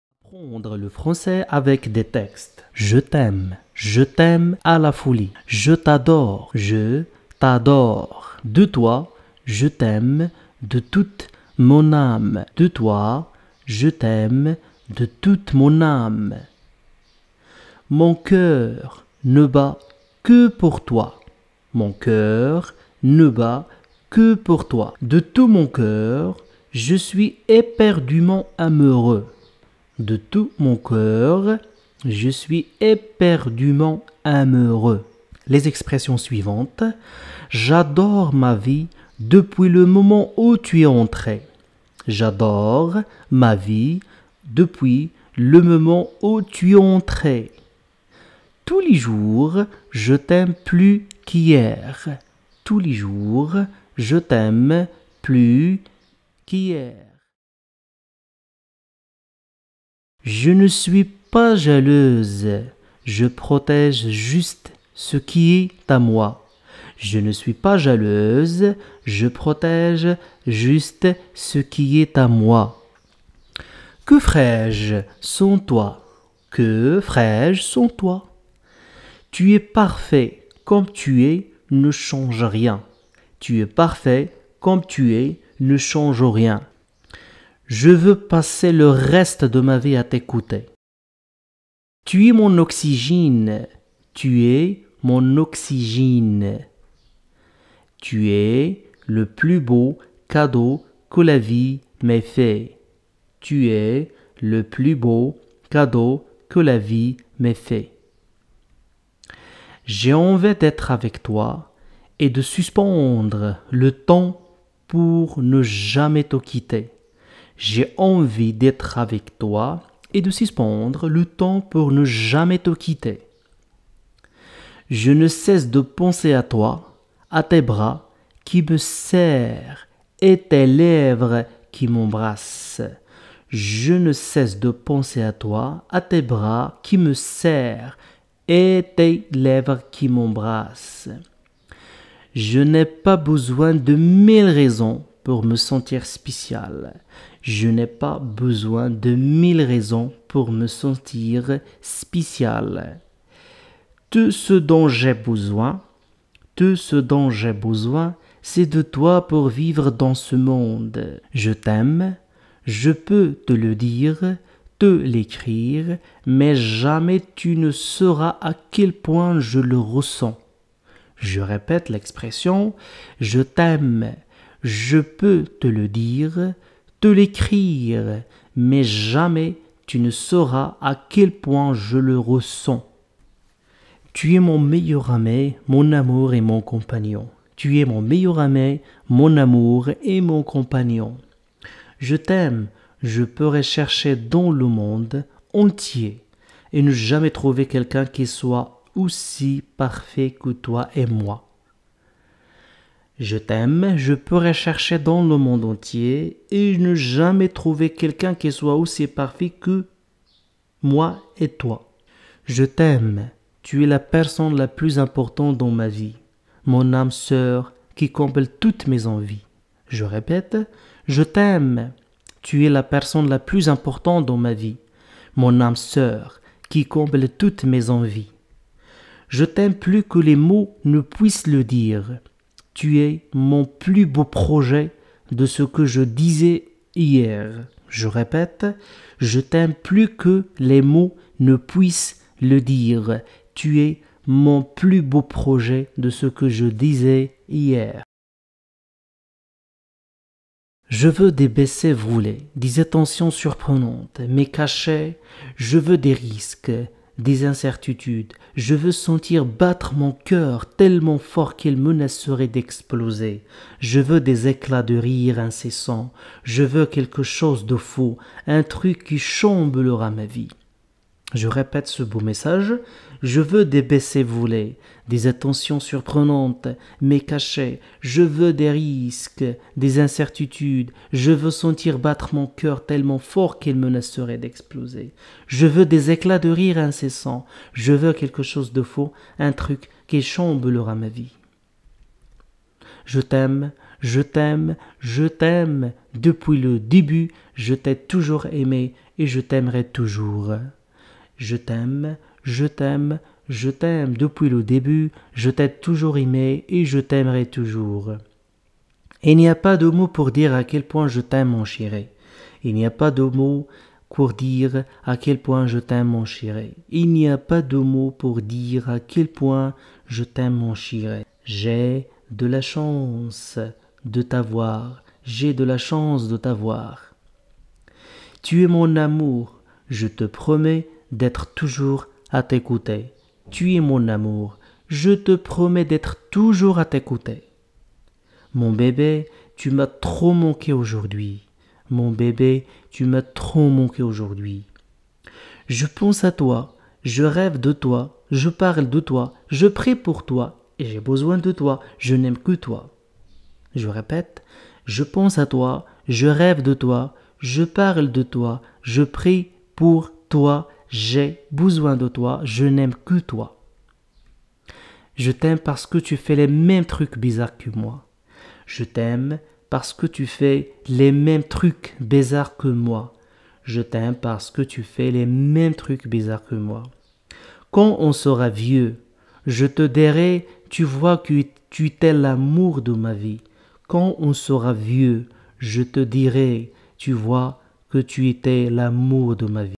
Apprendre le français avec des textes| learn French with texts
apprendre_à_lire_en_français, textes_faciles_en_français
Ces textes vont t'aider à apprendre le français à progresser et améliorer ta prononciation facilement.